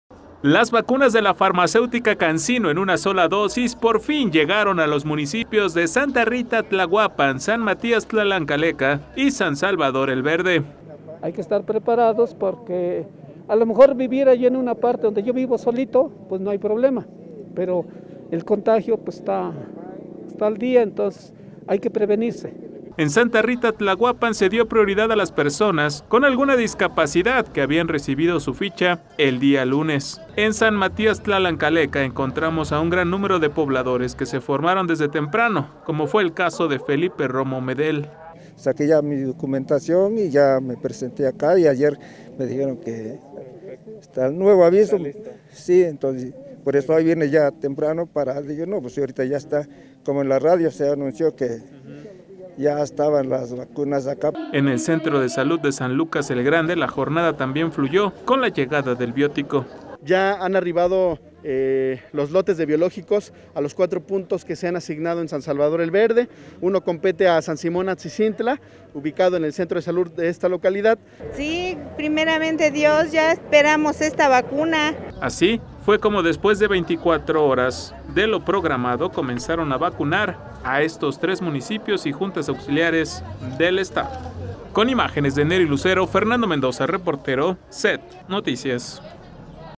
En San Salvador el Verde, Tlahuapan y Tlalancaleca llegaron las vacunas contra covid 19
LAS VACUNAS DE LA FARMACÉUTICA CANSINO EN UNA SOLA DOSIS, POR FIN LLEGARON A LOS MUNICIPIOS DE SANTA RITA TLAHUAPAN, SAN MATÍAS TLALANCALECA Y SAN SALVADOR EL VERDE